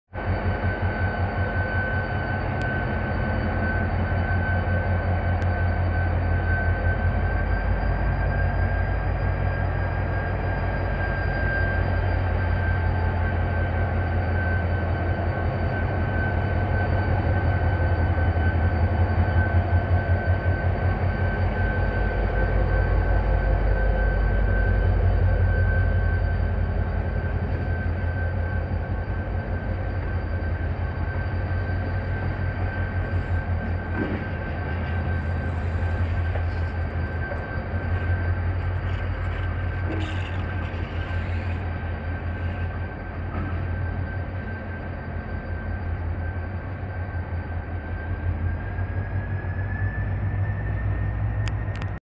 GT22 A918 (Ex lsm) trasladando GT22 9085 de Ferrobaires en Plaza C
En este video podran ver como se realiza el traslado de la locomotora GT22 9085 que pertenece a Ferrobaires a cargo de la locomotora GT22 A918 de Nuevos Ferrocarriles Argentinos ( Ex Linea San Martin) en la Plataforma 13 para ser llevada a un desvio dentro de Plaza Constitucion en la Linea Roca